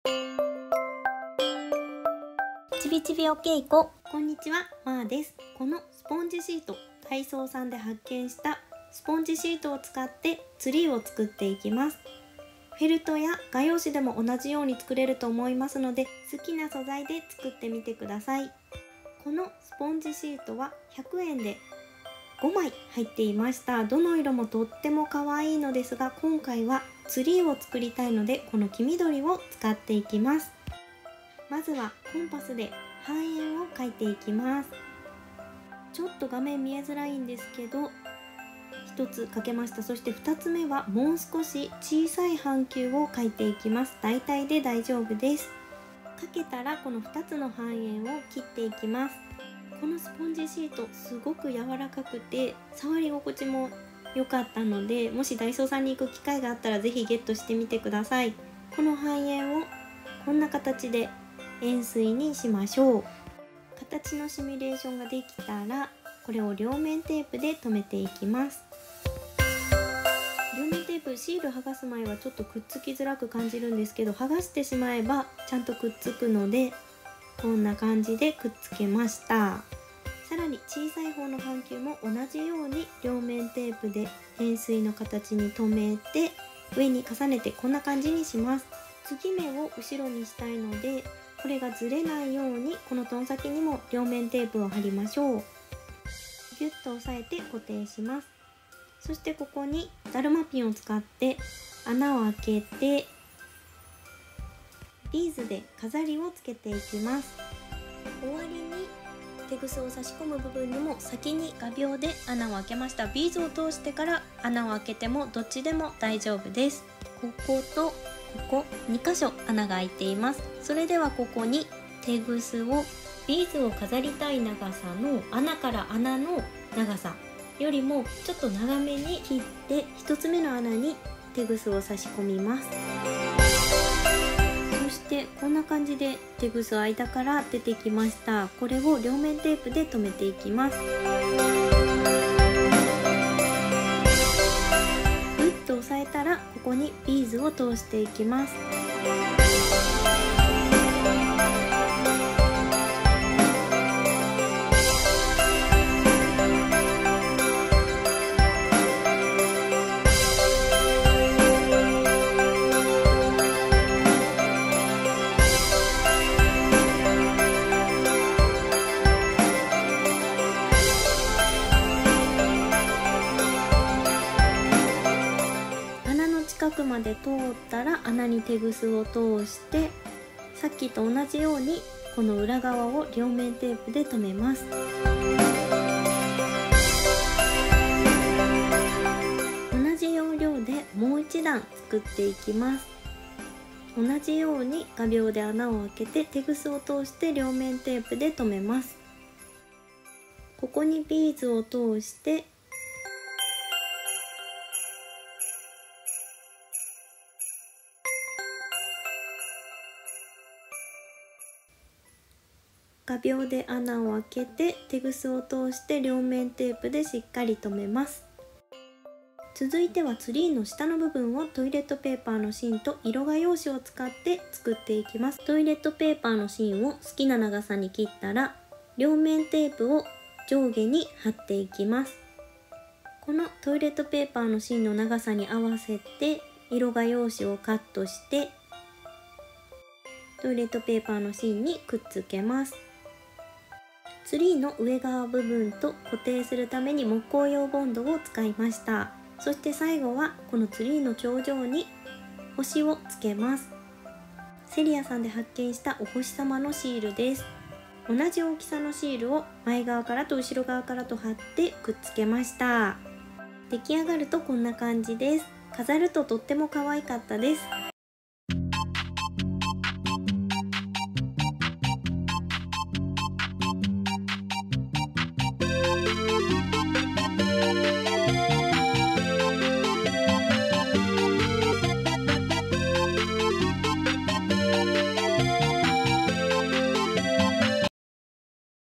【超簡単！知育にもなる⁉️】簡単でも手作り！親子で手作りは楽しい〜クリスマスツリー作り🎄こどもと作って飾ろう♪🎄
❣️🆕【知育にもなる！？クリスマスツリー作り】動画制作中❣️

【材料】
＊スポンジシート　ダイソー
＊テグス　セリア
＊ビーズ　セリア
＊両面テープ
＊トイレットペーパーの芯
＊色画用紙　セリア
＊はさみ、コンパスなど

【動画の内容】
　簡単に作れるのに失敗なし！とっても楽しい工作♫
　クリスマスパーティー🎄で飾りたい…そんなクリスマスツリーです💡
　材料もおうちにあるもの…なくても100均に行けば揃うものばかりです！

　工作しながら知育もできる！？🤗
　「なんで木の形（円錐）になるの？」「なんでまる〜く描けるの？（コンパス）」など…
　「なんでこうなるの？」子どもの疑問がどんどん出てくる！？

　クリスマス会で飾ろう〜🎉
　クリスマスまでにいっぱい作ろう〜♪

【関連動画】
❣️クリスマスパーティーで実験！？3連発❣️

【ちびちびおけいこ】
🎀Growing up with chibichibiokeiko🎀 を目指しています